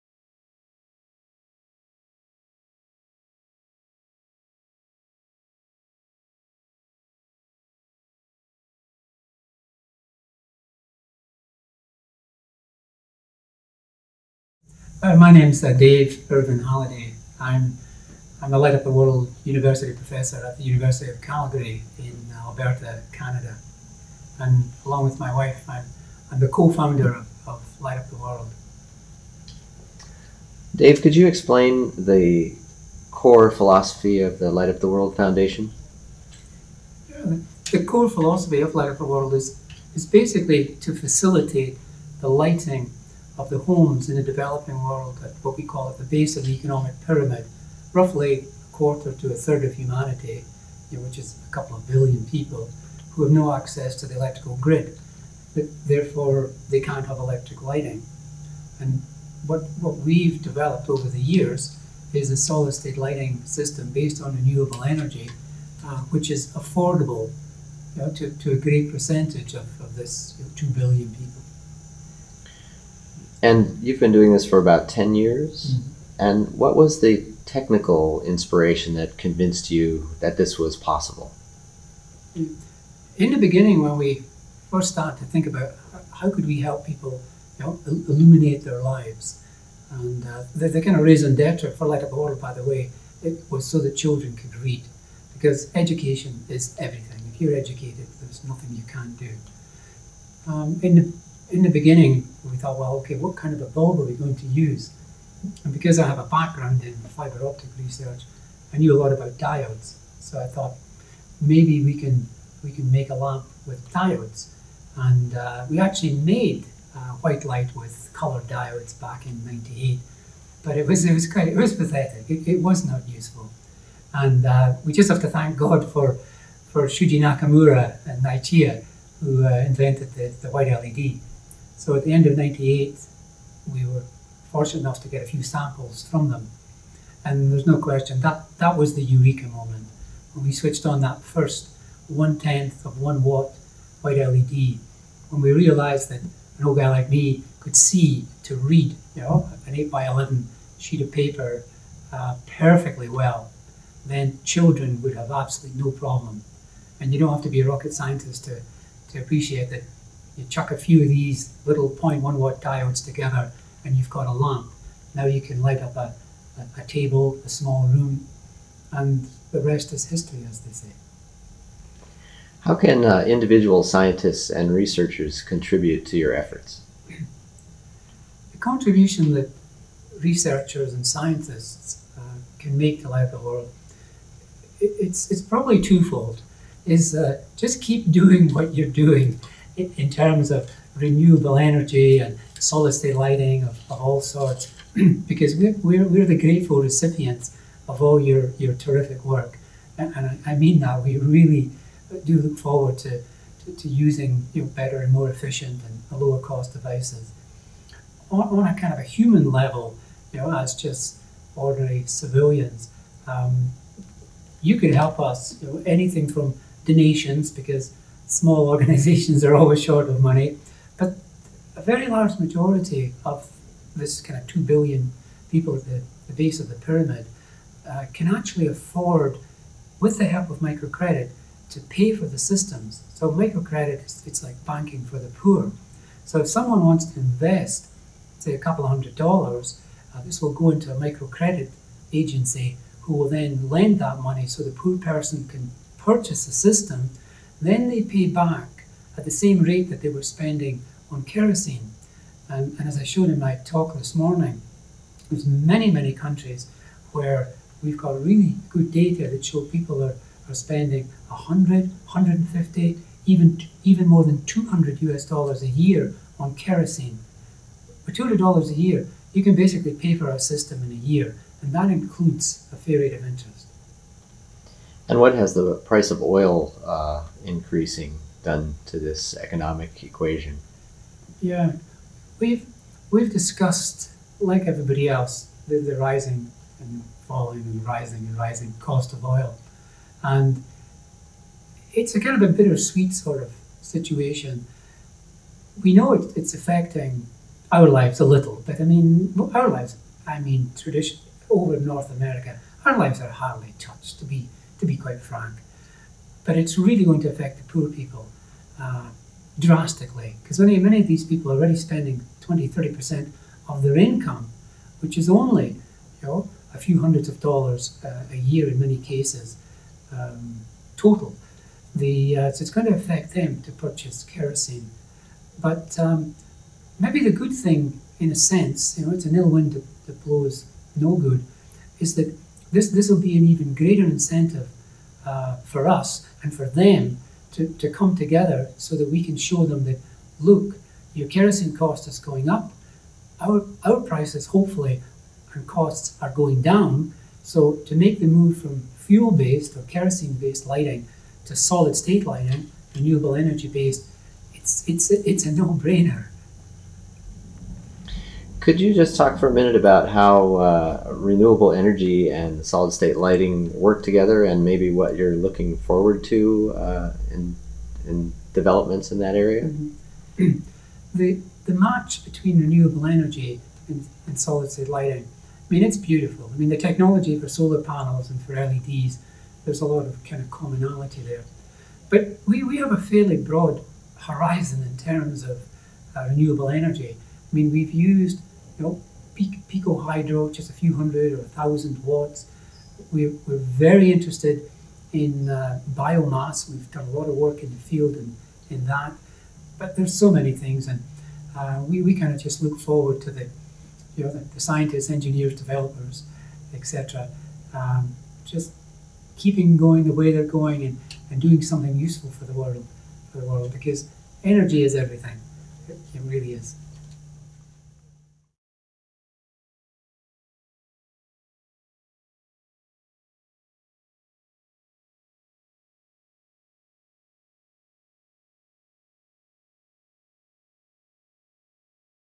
Dave Irvine-Halliday: Light Up the World
Light up the World (LUTW) is a not-for-profit organization that uses renewable energy and solid-state lighting to provide safe, efficient illumination in areas without access to power for adequate lighting. LUTW has proven the universal applicability of renewable energy-based SSL technology for people all over the world. In this interview for SPIE Newsroom, Professor Dave Irvine-Halliday of the University of Calgary, Canada, discusses his work with LUTW and how engineers can get involved.